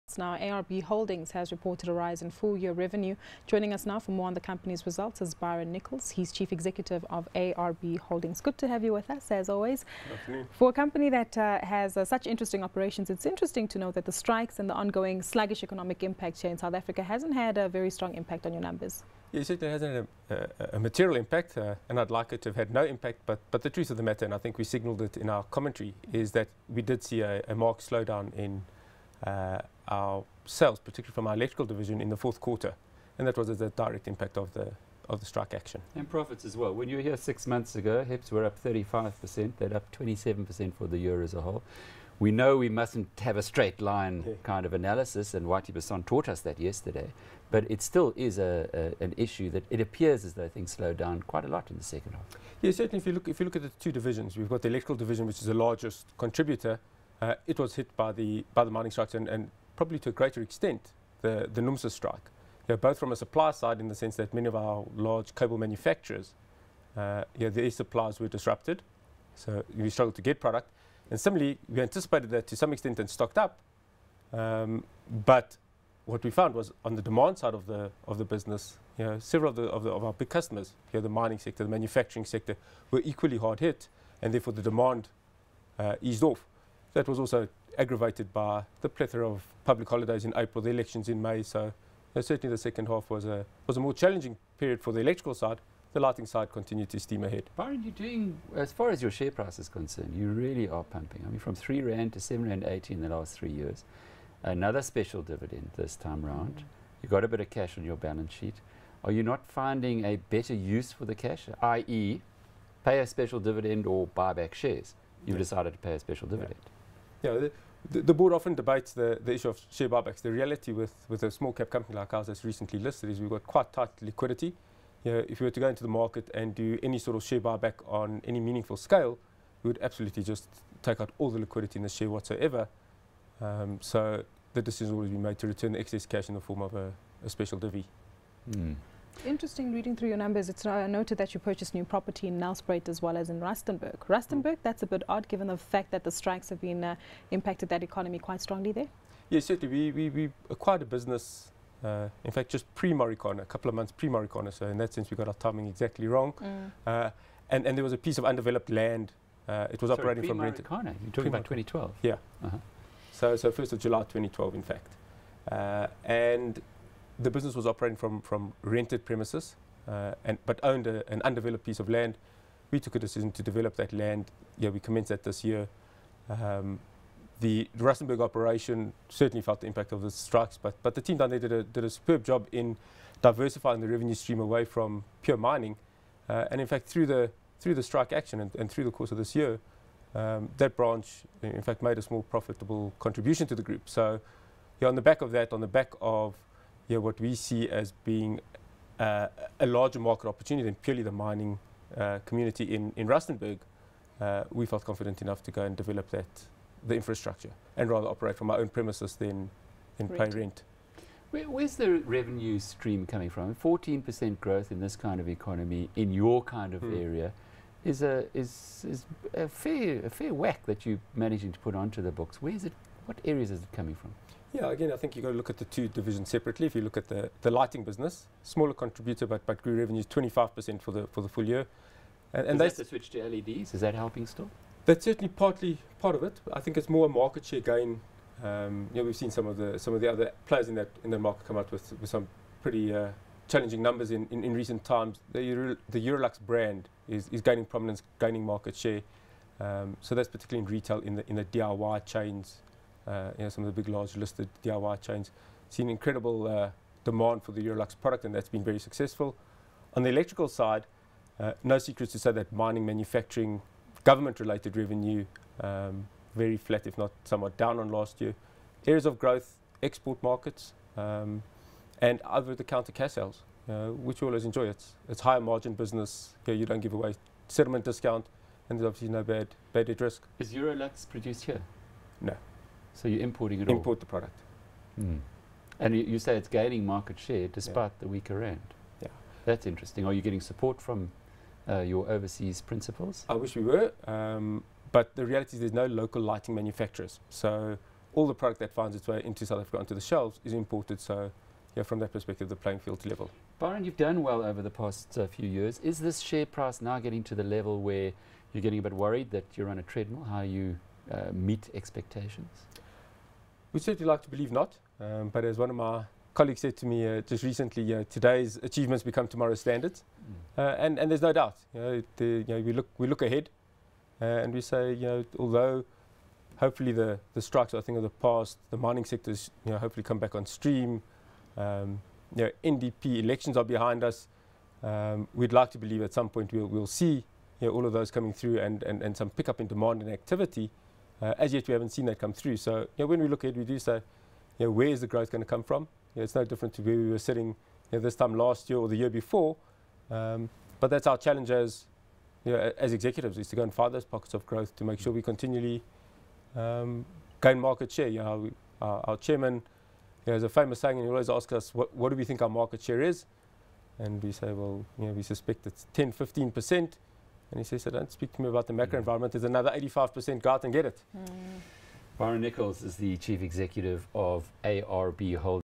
ARB Holdings reports a rise in full year revenue
Original Publish Date: || Wed, 20 Aug 2014 09:27:02 GMT || ARB Holdings has reported a rise in full year revenue, joining us for more on the company's results is Byron Nichles, CEO of ARB Holdings joins CNBC Africa for more.